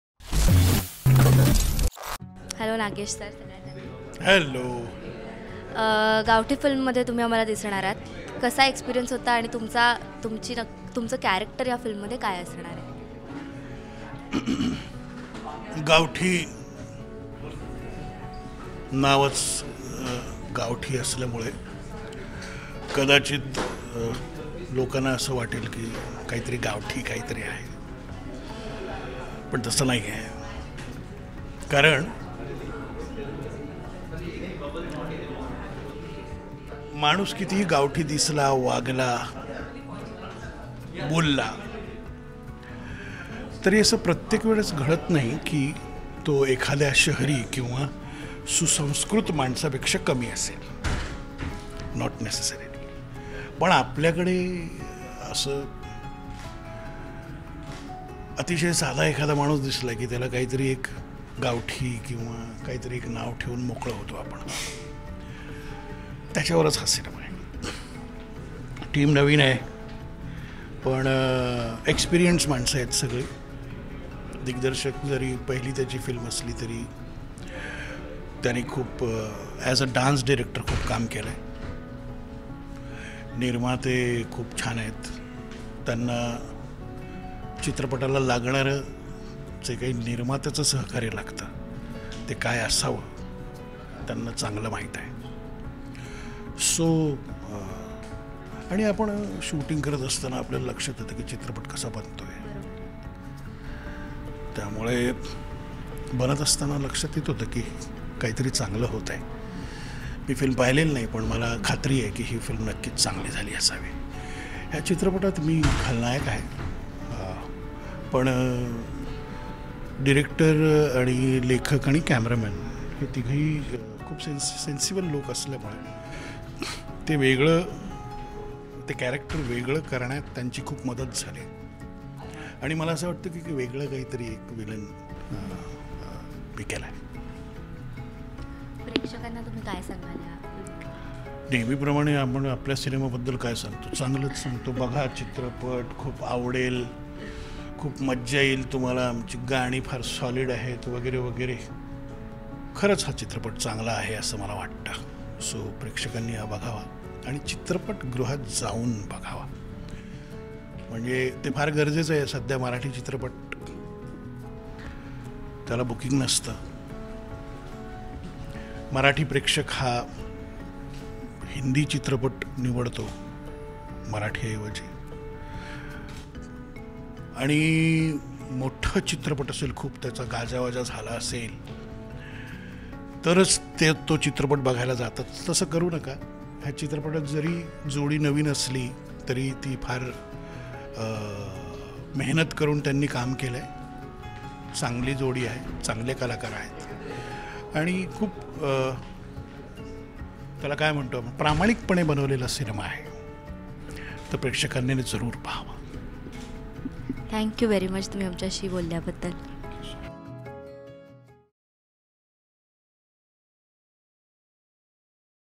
Gavthi गावठी | Interview With Nagesh Bhosale | Upcoming Marathi Movie 2018
Villain Of Gavthi Marathi Movie shares his experience about the movie.

Choreography, Screenplay & Direction Anand (Anddy)
Written by: Nayan Rathod & Rohit Sawant